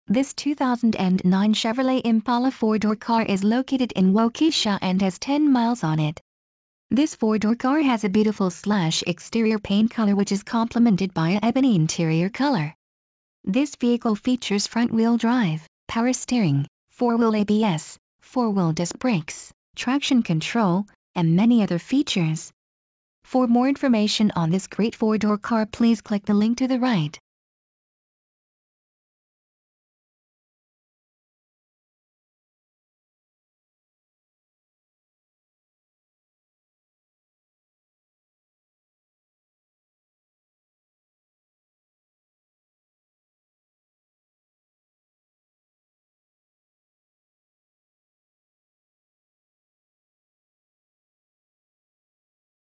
2009 Chevrolet Impala Milwaukee WI WI
Price: $ 26,892
Mileage: 10
Body Style: 4dr Car
Ext. Color: /
Int. Color: EBONY
Engine: Gas/Ethanol V6 3.9L/237
Drive Type: FWD
Fuel Type: Flex Fuel
Doors: 4
Stock No.: C90353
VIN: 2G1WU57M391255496